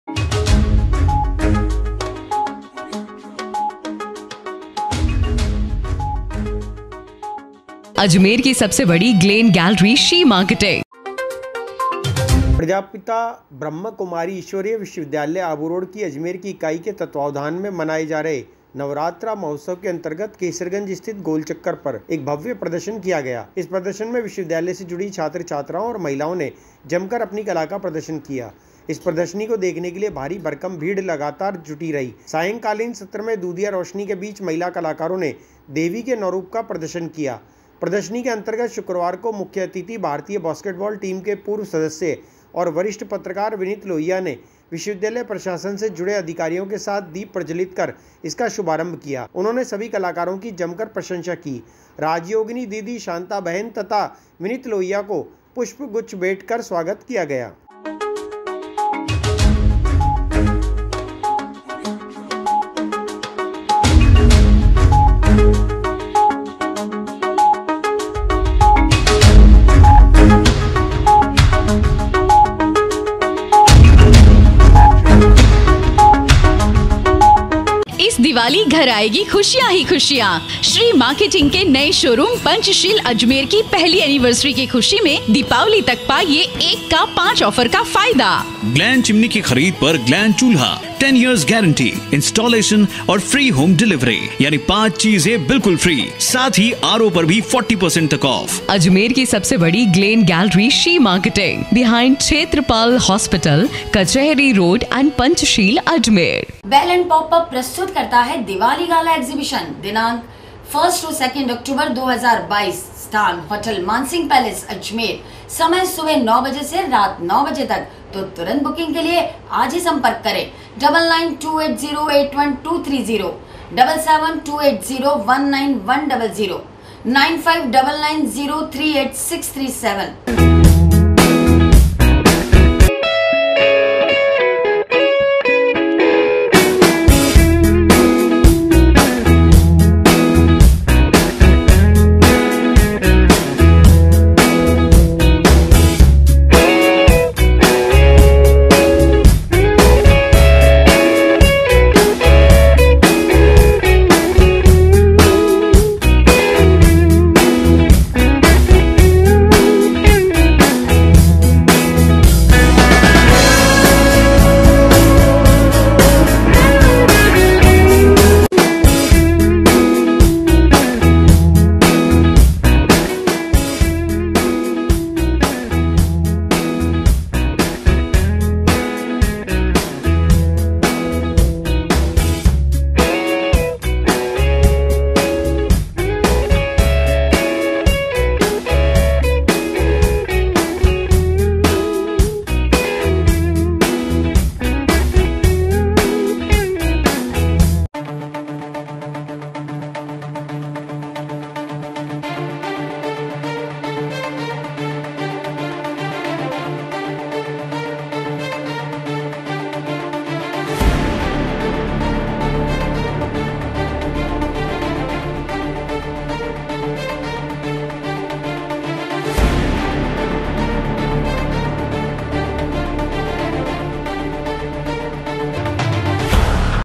महिला कलाकारों ने देवी के नौ रूप का किया प्रदर्शन II NEWS UPDATE II AJMER NEWS
महिला कलाकारों ने देवी के नौ रूप का किया प्रदर्शन  II NEWS UPDATE II AJMER NEWS प्रजापिता ब्रह्माकुमारी ईश्वरीय विश्वविद्यालय आबूरोड की अजमेर की इकाई के तत्वाधान में मनाए जा रहे नवरात्र महोत्सव के अंतर्गत केसरगंज स्थित गोल चक्कर पर एक भव्य प्रदर्शन किया गया इस प्रदर्शन में विश्वविद्यालय से जुड़ी छात्र छात्राओं एवं महिलाओं ने जमकर अपनी कला का प्रदर्शन किया इस प्रदर्शनी को देखने के लिए भारी भरकम भीड़ लगातार जुड़ी रही सांय  कालीन सत्र में दूधिया रोशनी के बीच महिला कलाकारों ने देवी के नौ रूप का प्रदर्शन किया!
  प्रदर्शनी के अंतर्गत शुक्रवार को मुख्य अतिथि भारतीय बास्केटबॉल टीम के पूर्व सदस्य एवं वरिष्ठ पत्रकार विनीत लोहिया ने विश्वविद्यालय प्रशासन से जुड़े अधिकारियों के साथ दीप प्रज्वलित कर इसका शुभारंभ किया उन्होंने सभी कलाकारों की जमकर प्रशंसा की!! राजयोगिनी दीदी शांता बहिन तथा विनीत लोहिया को पुष्पगुच्छ भेंट कर स्वागत किया!०